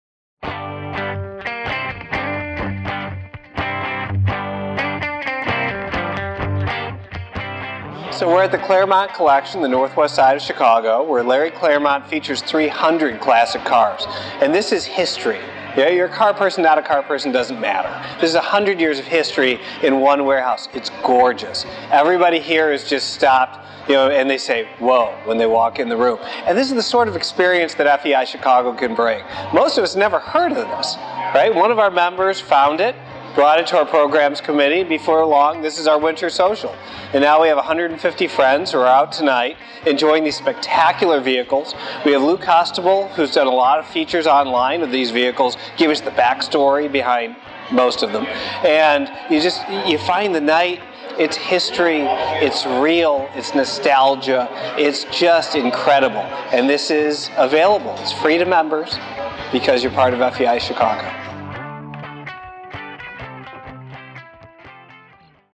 2016 Holiday Party at Klairmont Kollections - FEI Chicago
The FEI Chicago Chapter's Holiday Party was Tuesday, December 6, at Klairmont Kollections (Larry Klairmont's private car museum), located at 3111 North Knox Avenue in Chicago amongst 300+ classic cars in a private collection spanning over 100,000 square feet.

FEI Chicago
216 W Jackson Blvd, Suite 625
Chicago, IL 60606